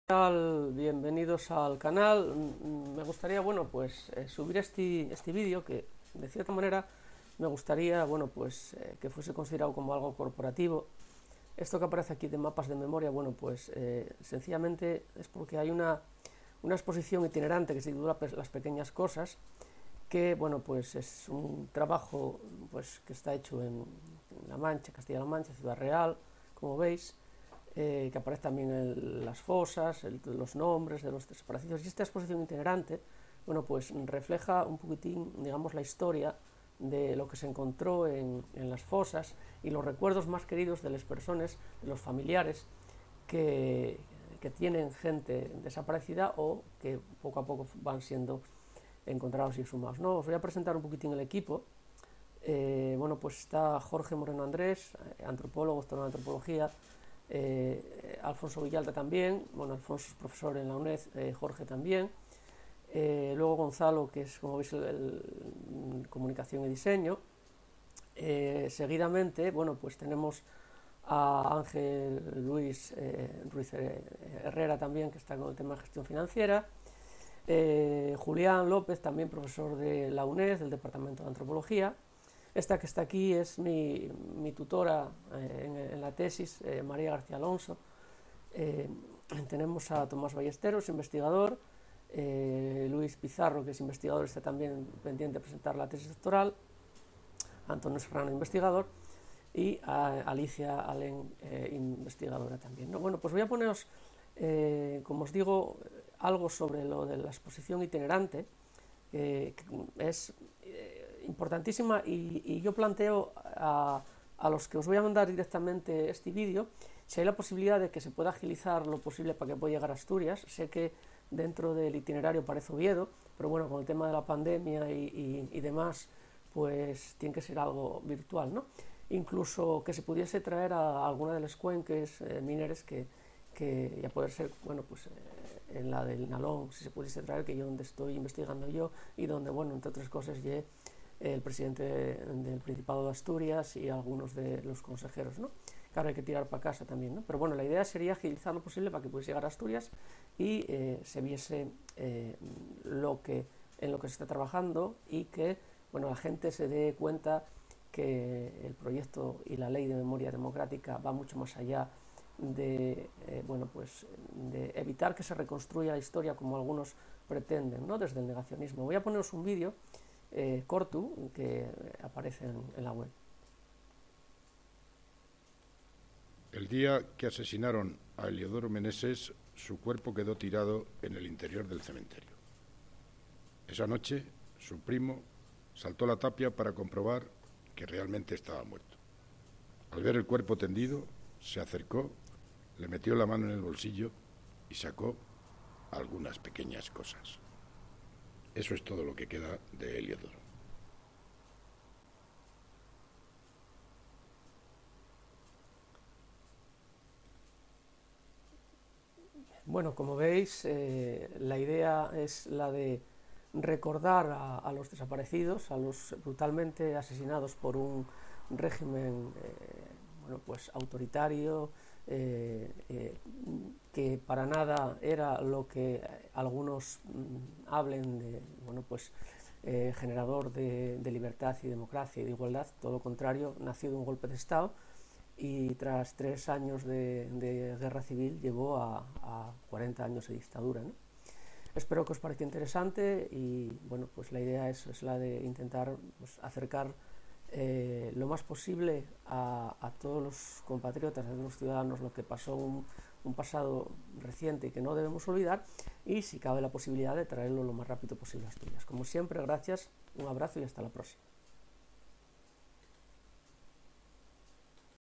Exposición itinerante "Las pequeñas cosas"
Se trata de presentar la exposición itinerante del Equipo de Trabajo del Mapa de Fosas de Ciudad Real, titulado "Las pequeñas cosas". La idea es acercar a los ciudadanos la Historia de los asesinados durante el franquismo para ayudar a su recuperación y para que sus familiares cierren de alguna manera un itinerario de dolor de más de ochenta años en algunos casos